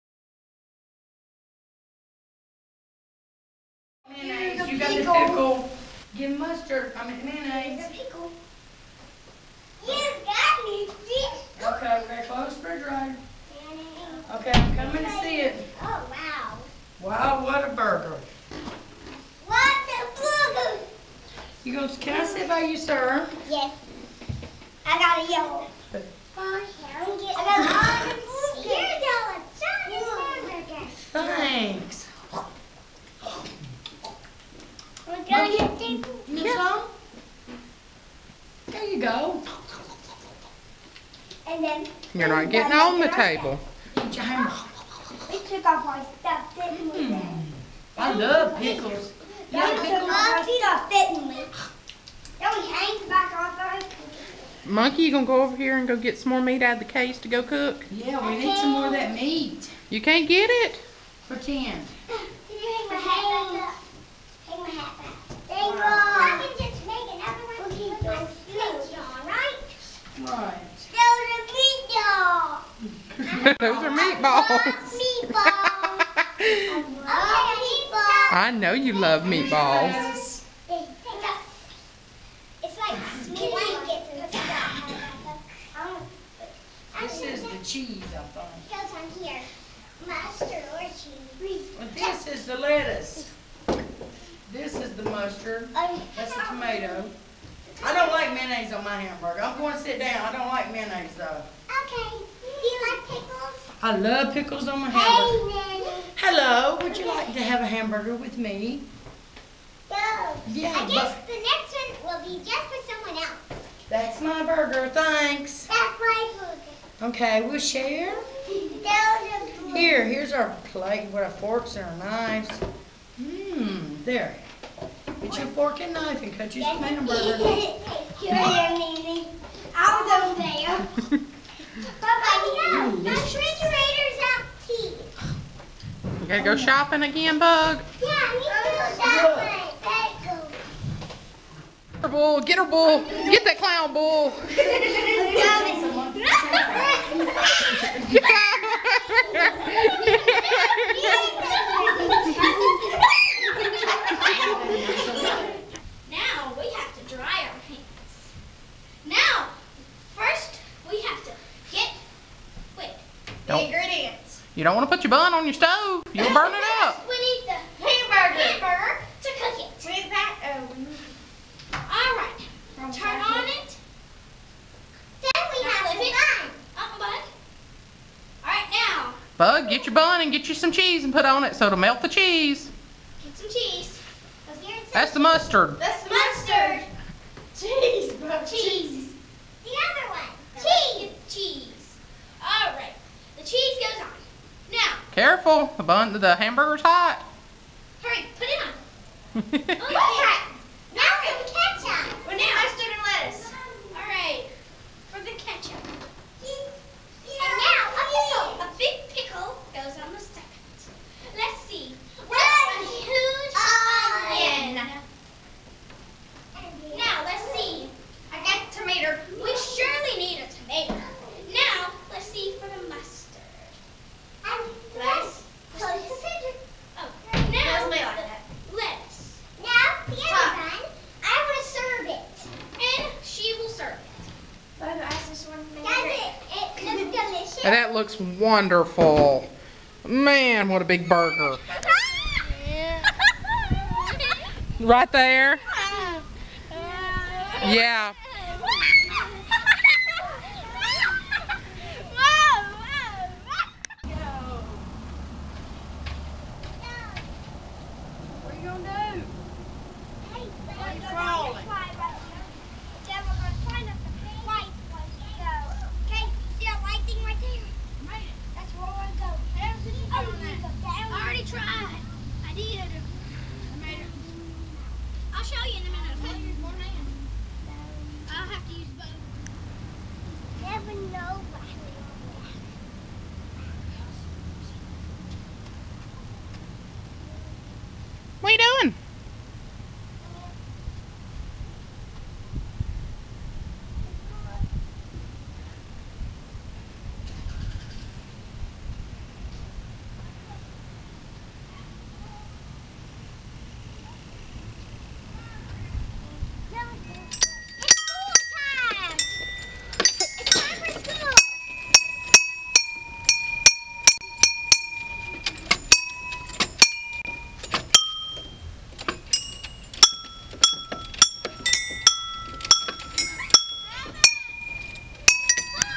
Downtown Montgomery Outing for Cadie's Birthday
We took the kids to the MOOseum, the History Museum, and to the park for Cadie's birthday. 9/26/11